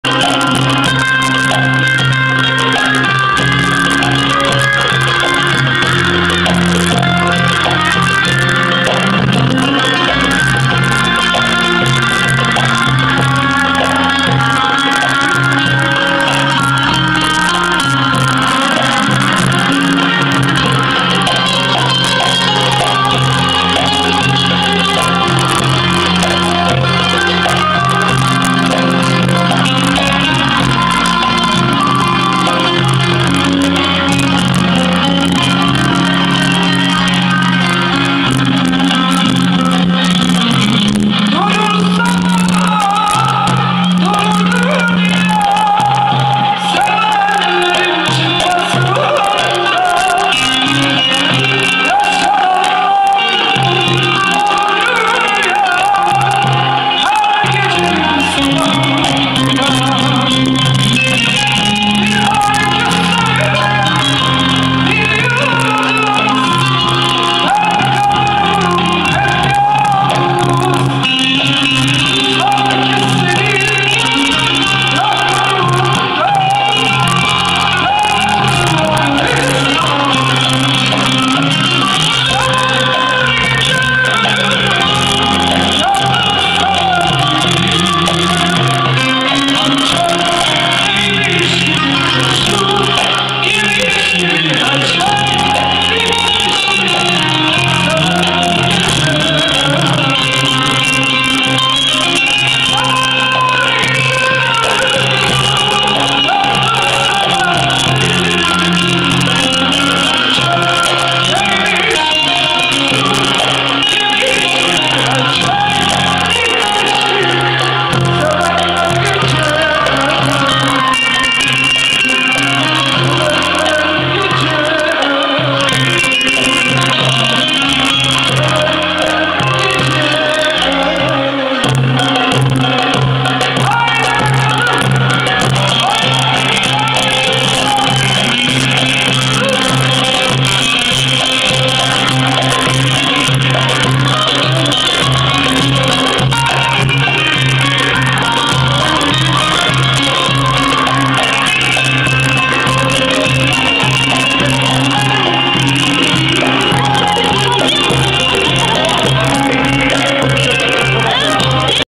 Kayseri
Mıstılının Ali Emmi nin Torunu Muammer Ülker in oğlu Hüseyin in Düğünü   Real Düğün Salonu  kayseri 11